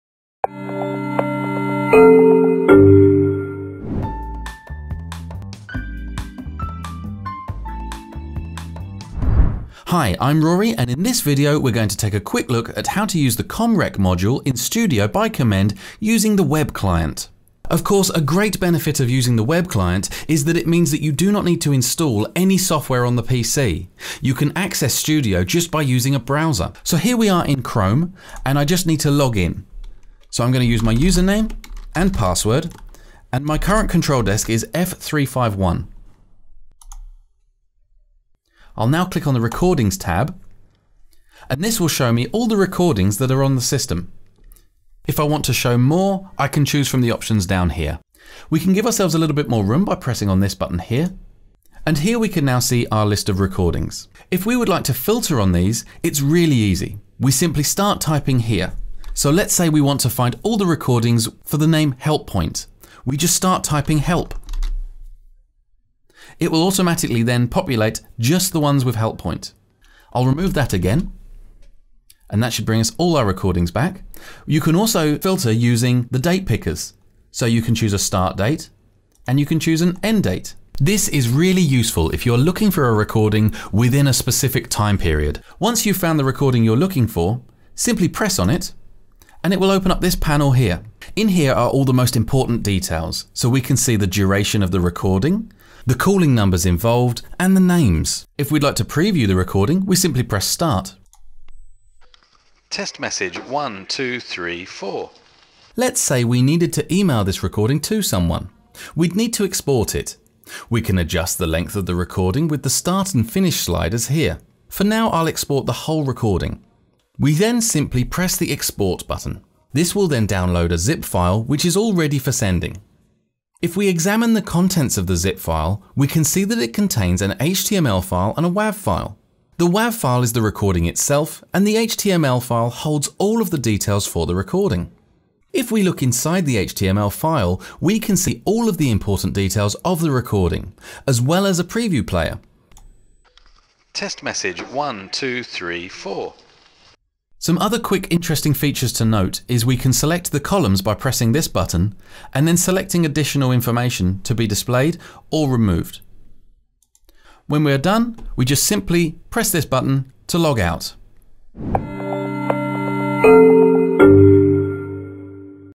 Studio Shorts - Accessing Recordings in the Web Client
Episode 1 - In this Studio Short video we take a look at how easy the workflow is for accessing audio recordings created by Studios's ComREC module using the Web Client.

Please let us know if you have any feedback.